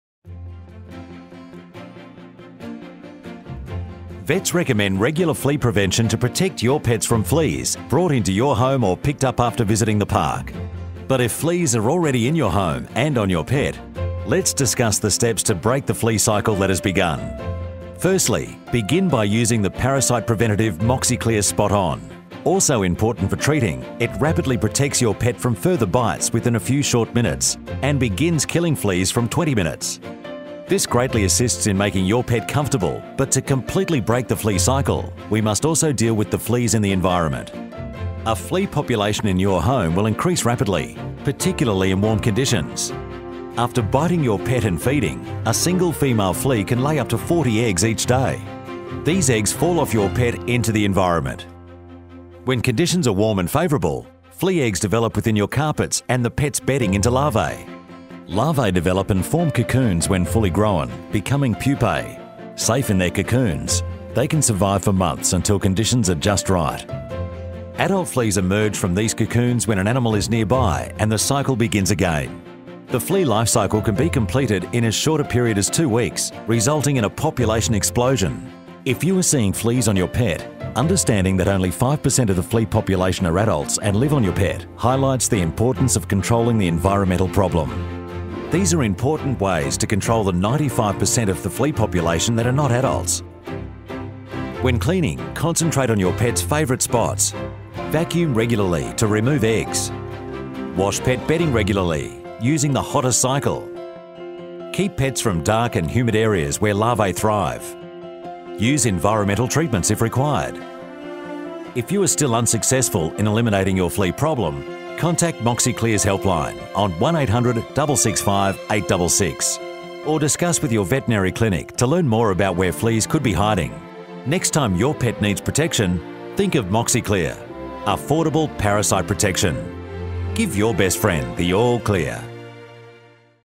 MOXICLEAR Understanding the Flea Lifecycle
This video contains important information about the flea lifecycle and how this influences the treatment and management of pets with fleas.
This video is intended for Australian residents.